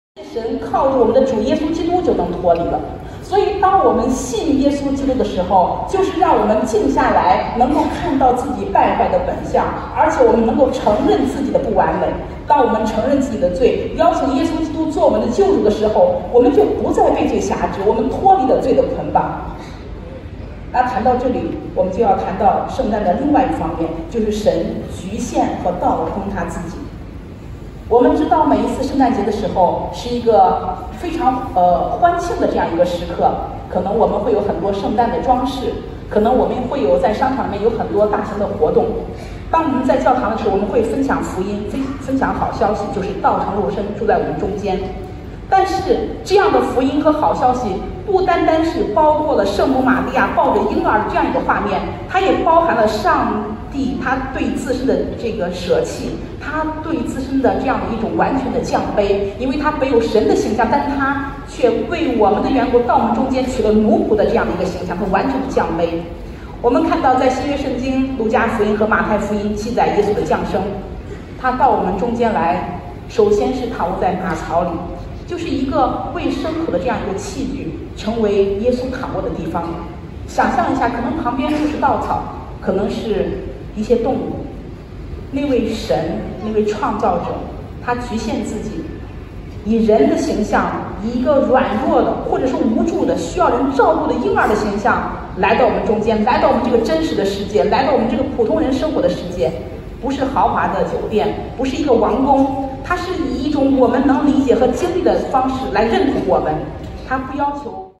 Sermon by Pastor He Yonghua贺永花牧师的证道｜Christmas Eve Service平安夜圣乐崇拜｜Haidian Christian Church北京基督教海淀堂
Recorded December 24, 2023 by Adam Ensign in Beijing, China